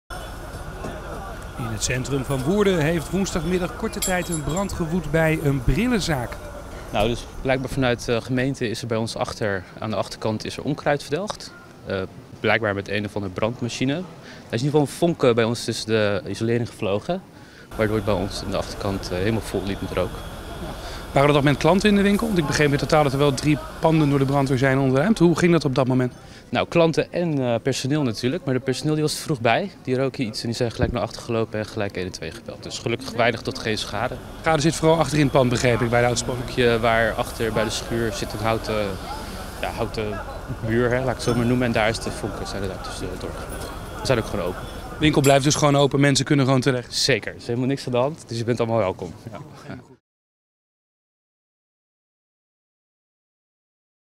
Woerdense brillenzaak vol rook, mogelijk door onkruidverbrander | Woerden.TV
Bekijk het nieuws uit Woerden en omgeving.

Wij plaatsen video's uit Woerden, Harmelen, Kamerik, Zegveld, Montfoort, Linschoten, Oudewater.